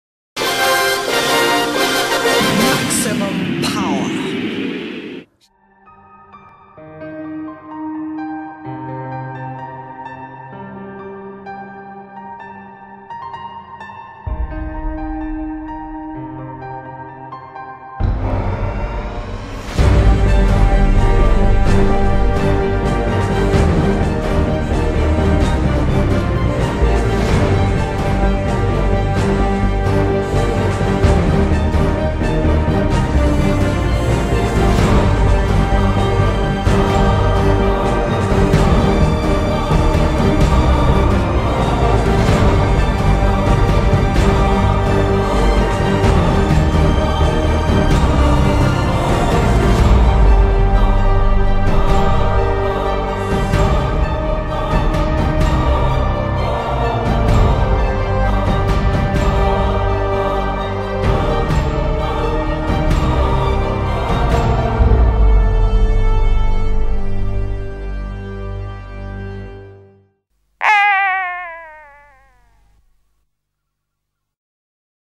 Maximum Power Jan Chappell
'Maximum Power', a Blake's 7 event in aid of Marie Curie. Sunday 23rd June 2019, Steventon, Oxfordshire. Special Guests, Original Costumes & Props, Displays and lots more.  Celebrating Blake's 7 and in honour of Jacqueline Pearce. Tickets are now on sale.